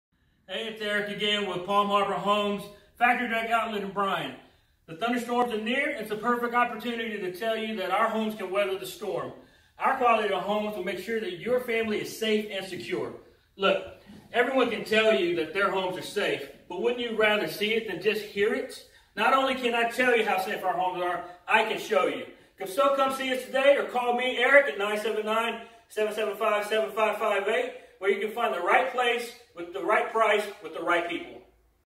$15,000 off select double wides! Palm Harbor Factory Direct Outlet Bryan TX
Often imitated but NEVER duplicated! Everyone says their homes are built the best and are safe but do they ever show you how or why? As they say, the proof is in the build and talk is talk. Come let Eric SHOW you how we are built better! It’s what’s underneath all the fancy stuff that counts!